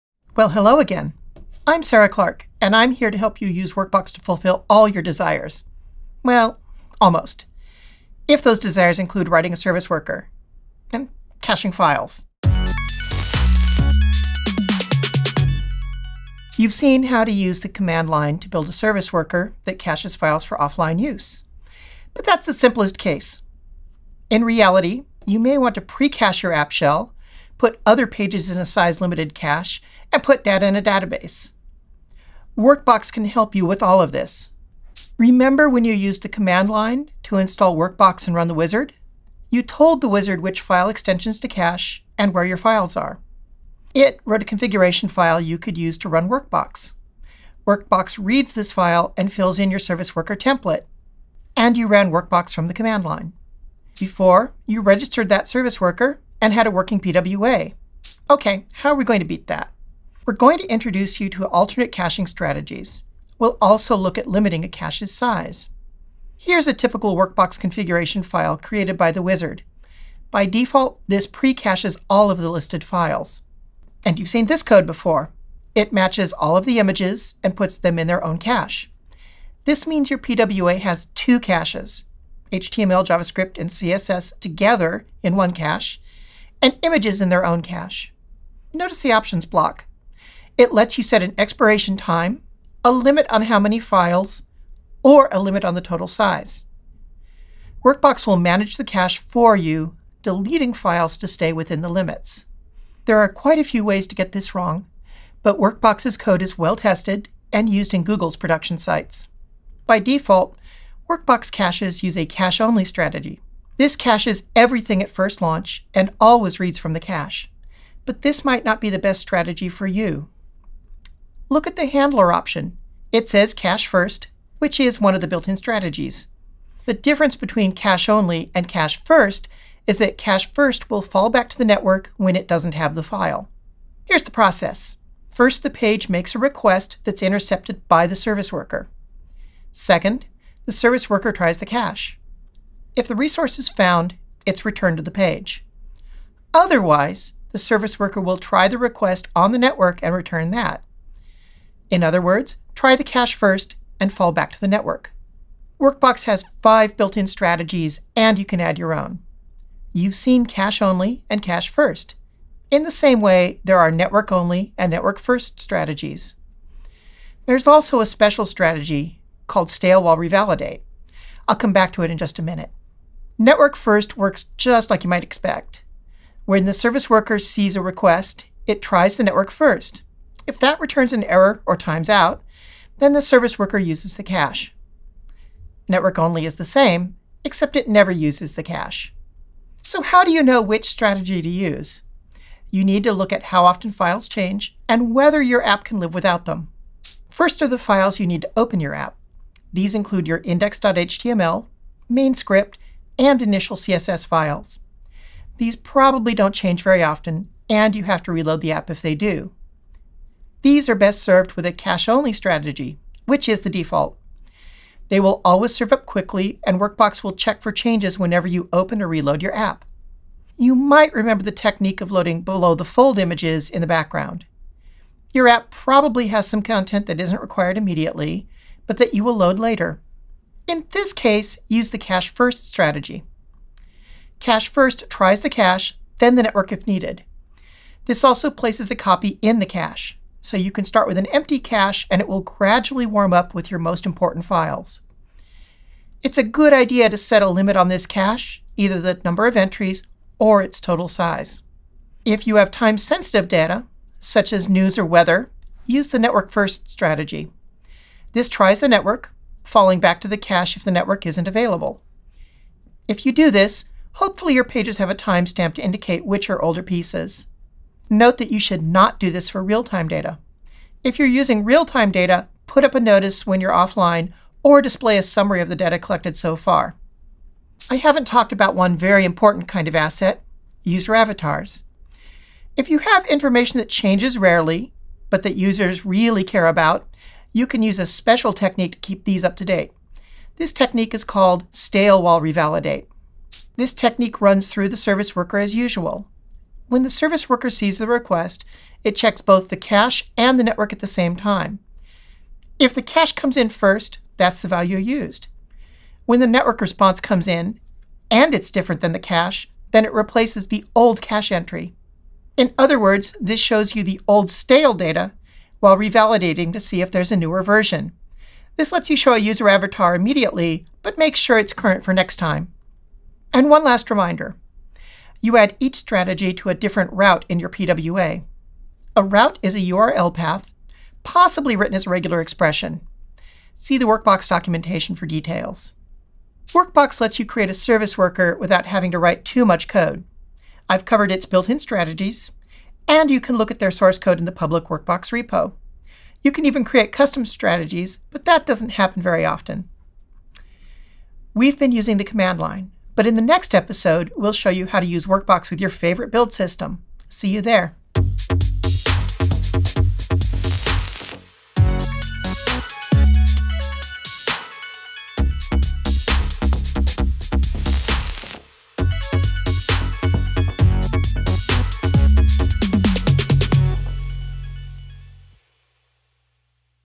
Using Workbox to manage your caches - Progressive Web App Training
How can you pre-cache your core app resources, put other pages in a size-limited cache, and store data in a database?

Workbox can help you with all of this.

This video shows you how.